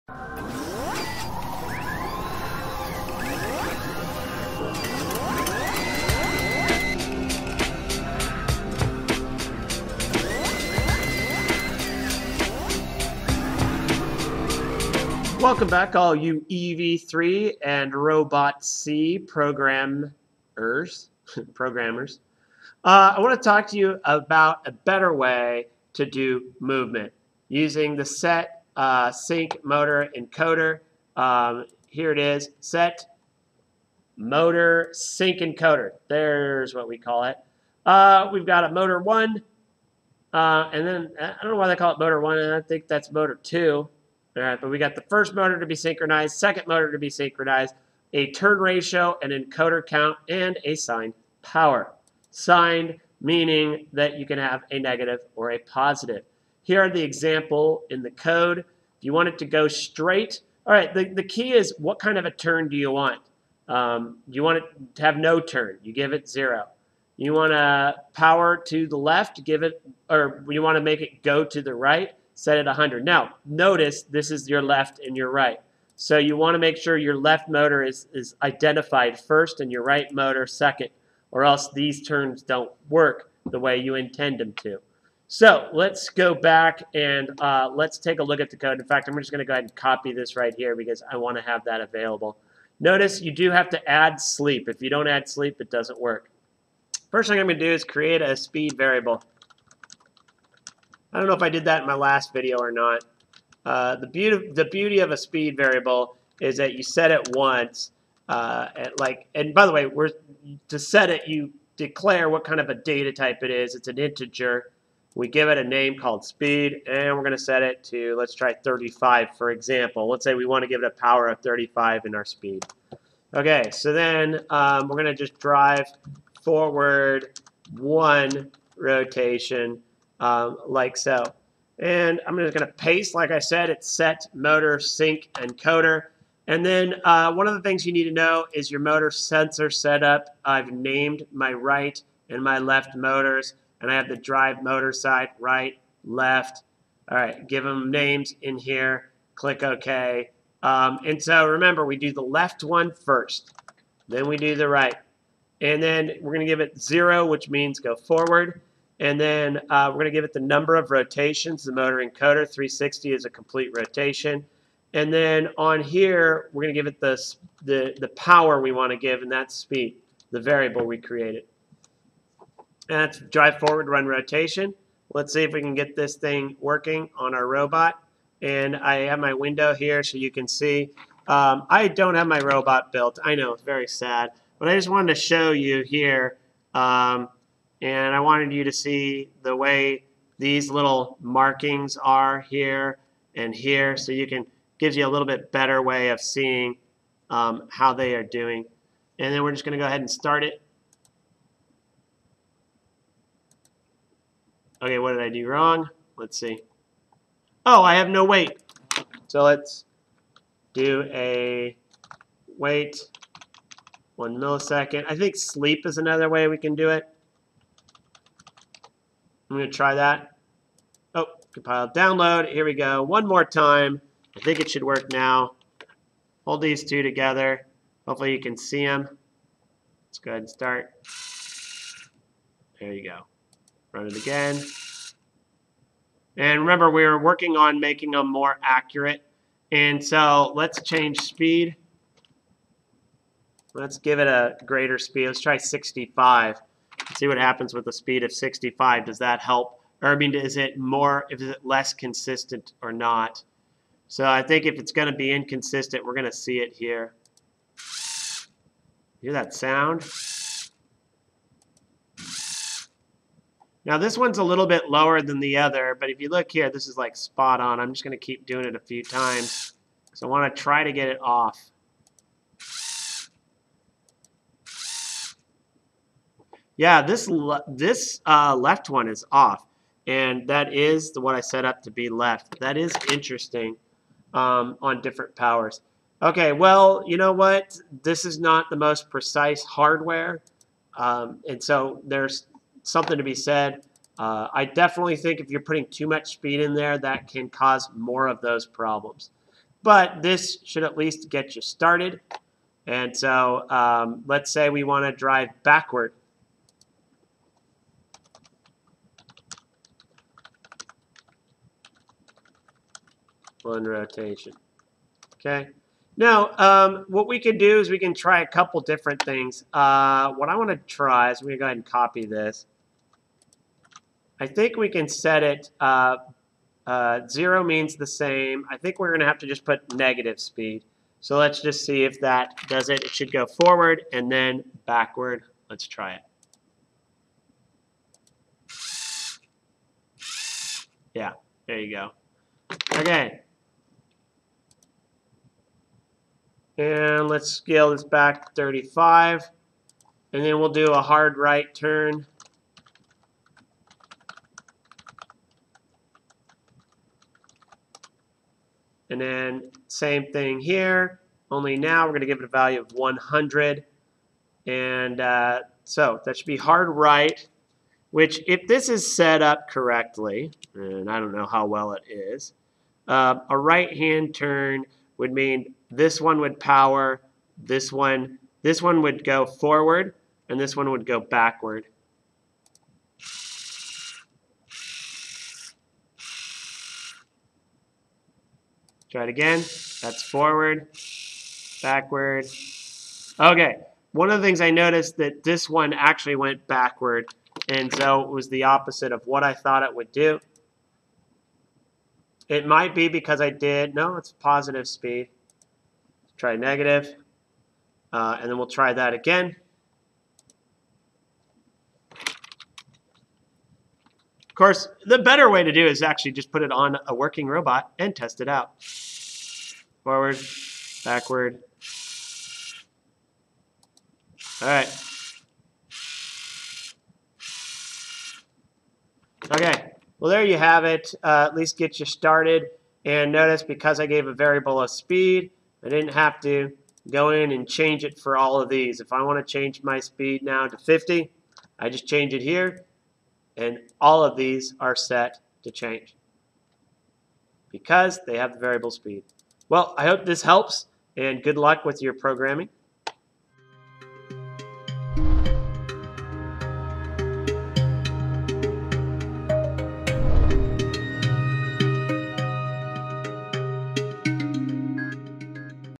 Programming the Motor Encoder for EV3 Movement Part 2
This video shows how you can more accurately program your EV3 motors to move in sync using the motor encoders, and that's through the setMotorSyncEncoder() function.